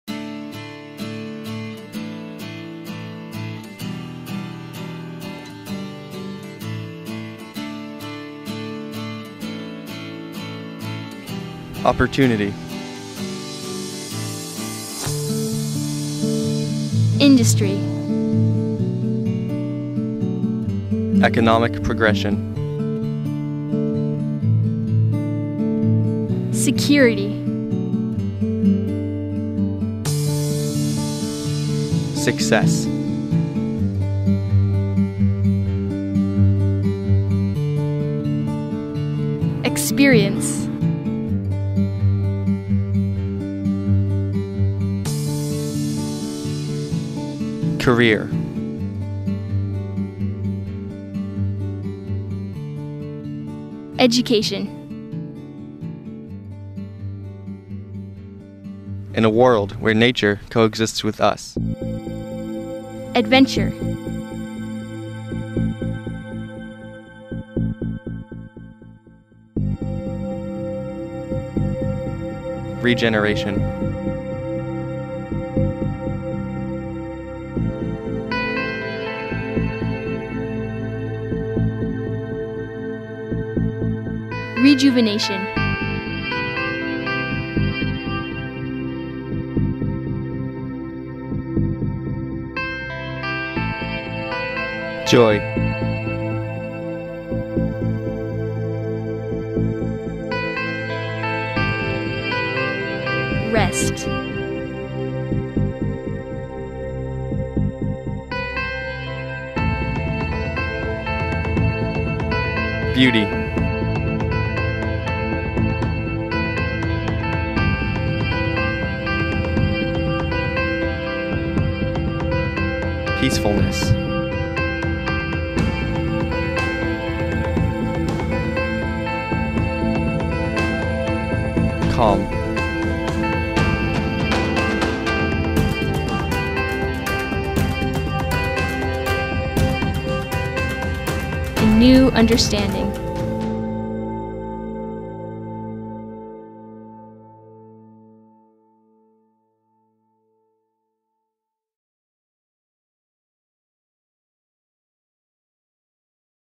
BC Skills Competition 2013 Provincials (Bronze)
This was made in the skills comp. in BC. We didn't have a lot to work with, so bronze was really cool! :D Our theme was the words "beautiful BC". But that was kinda hard when you're at an airport and a trade center, and the only forest is fenced off. XD